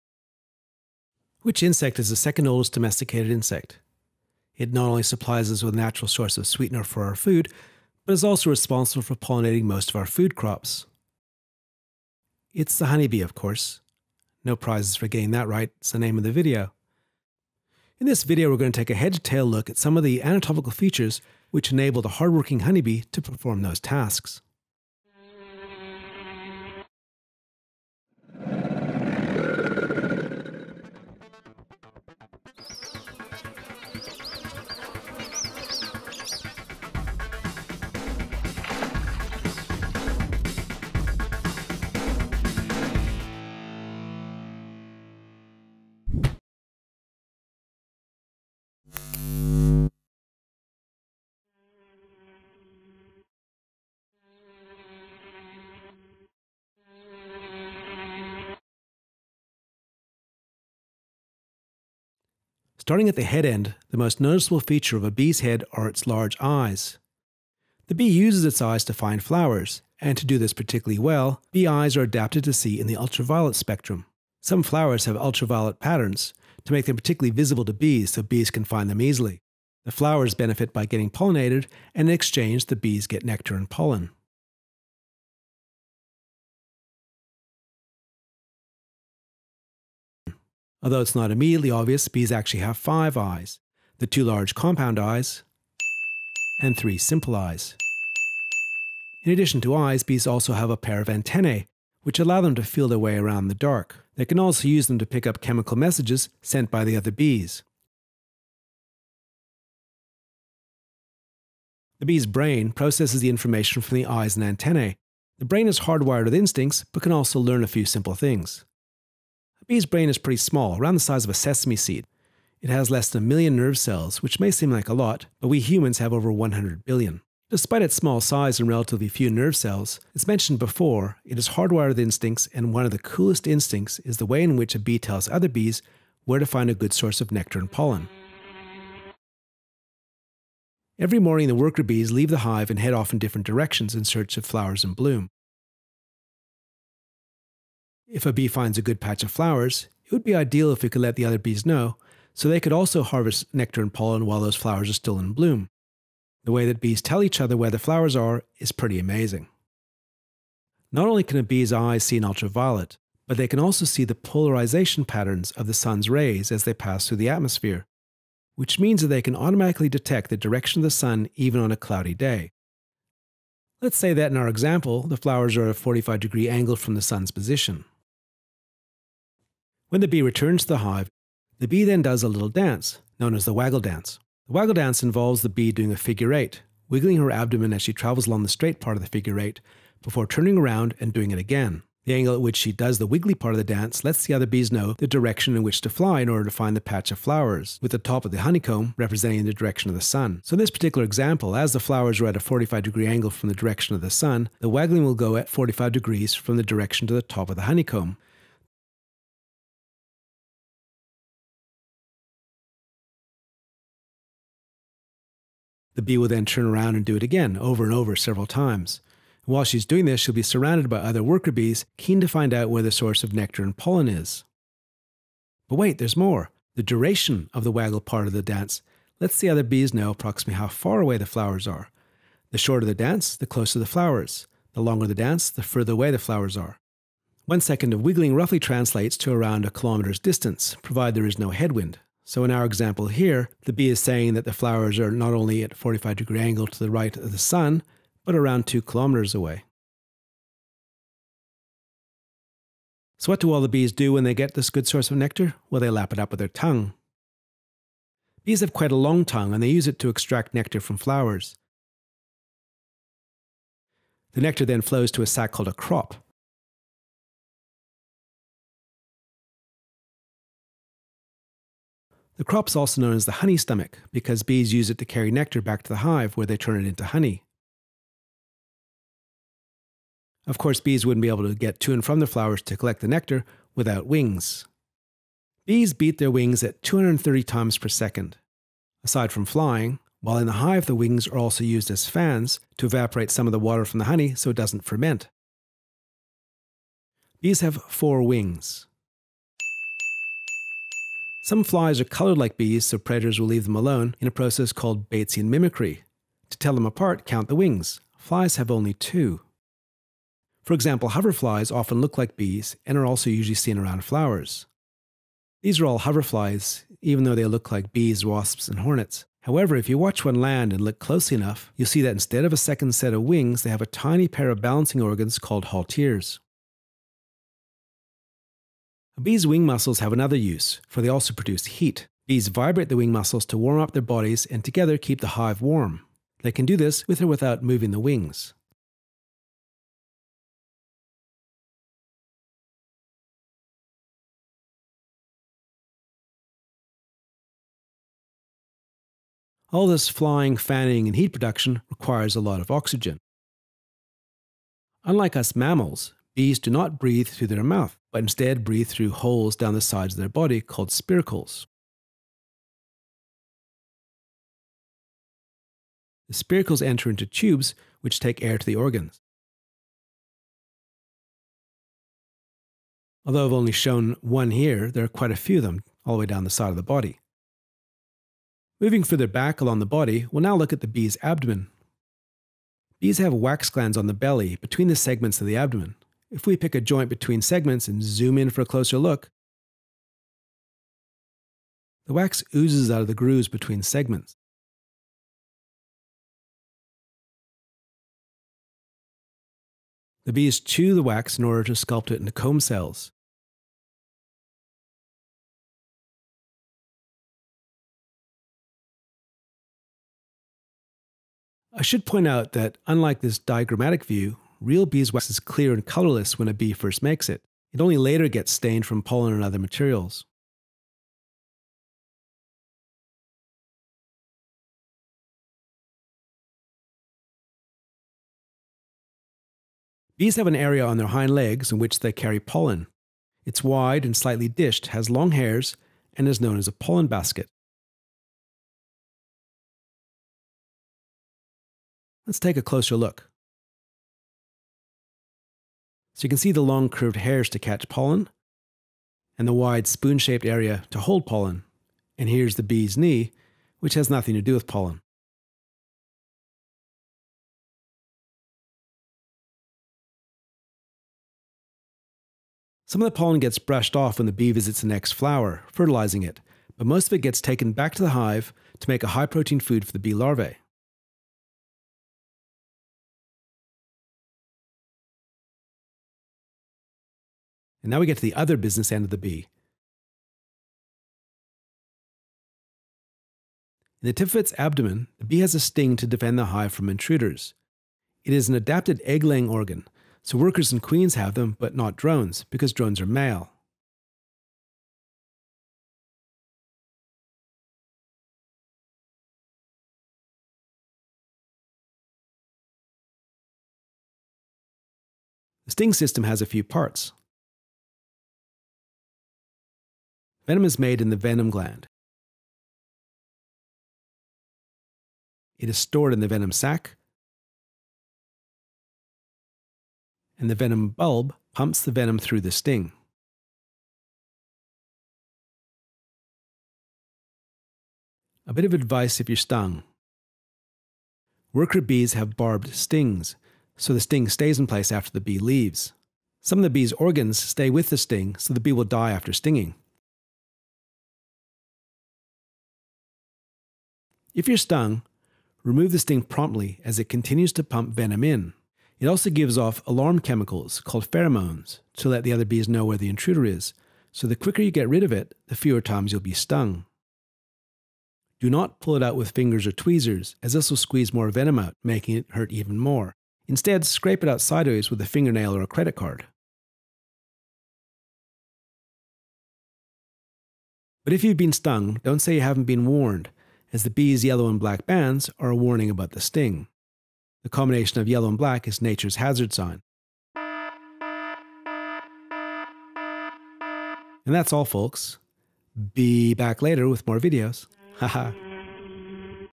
Bee Bits
An animated, head to tail look at the functional anatomy of the worker honeybee.

Hoverfly images: Alvegaspar (source: wikicommons)